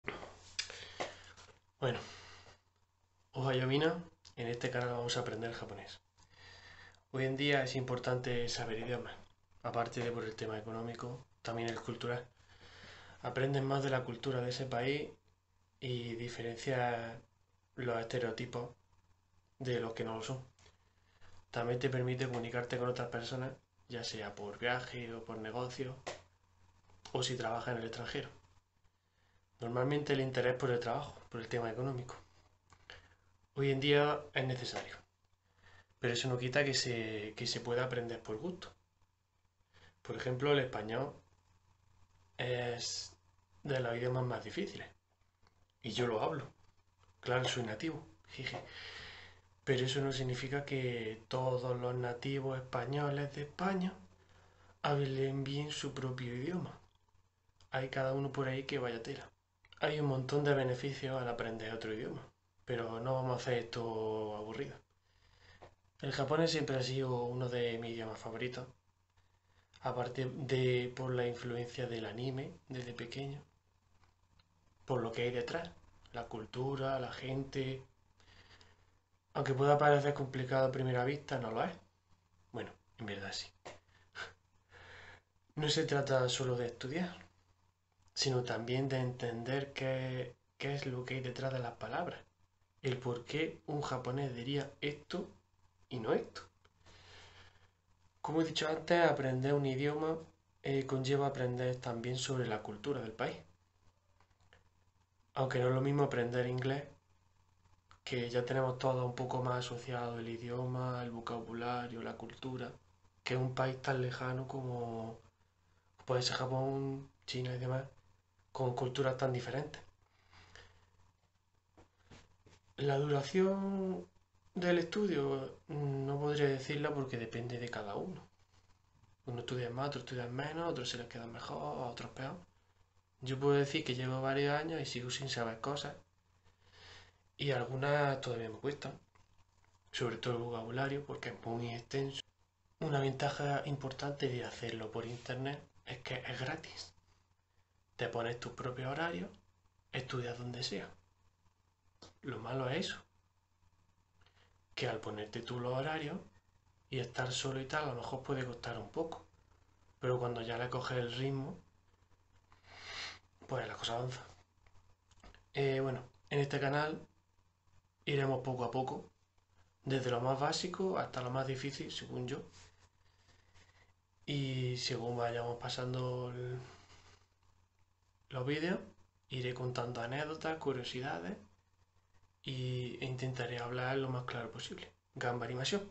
Introducción
Esto es una introducción mía y al canal (para quitarme la vergüenza más que nada).